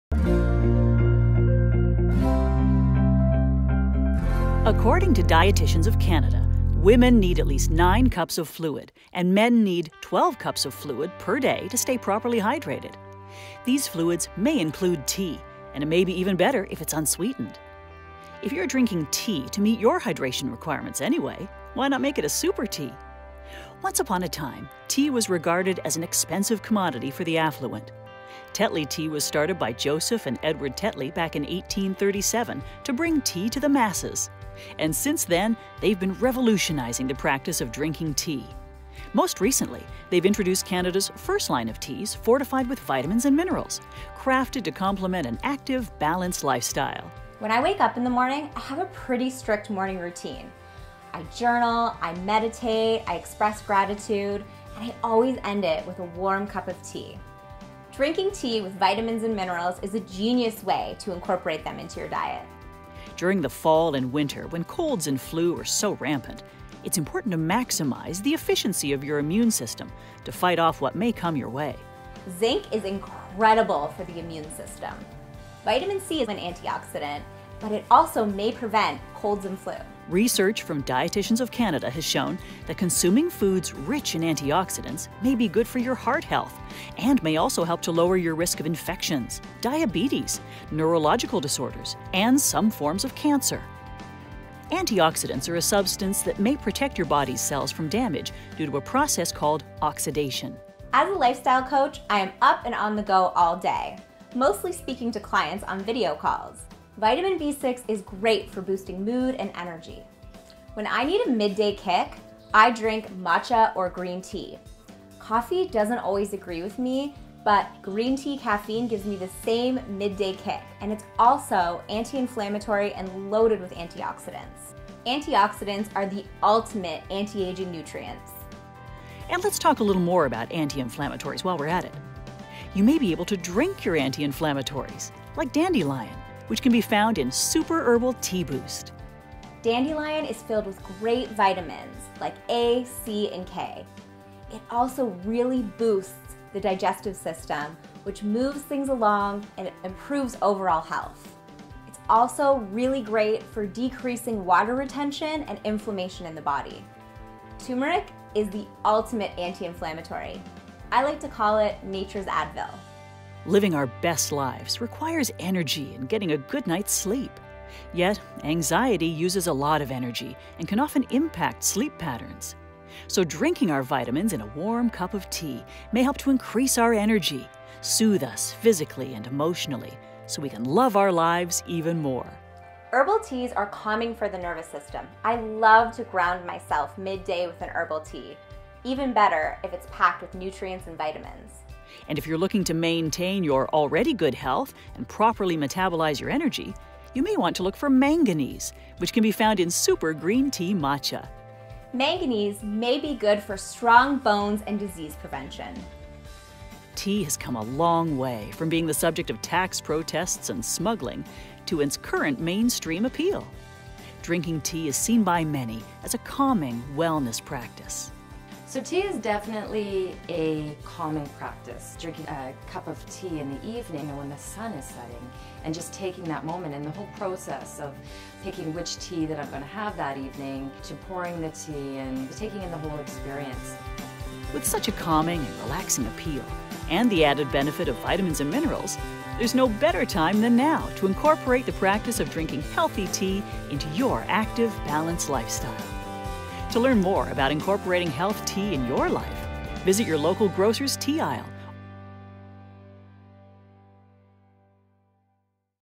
Drinking Tea with Benefits
Watch how you can incorporate Super Teas into your diet to help boost your immune system and get your anti-inflammatories daily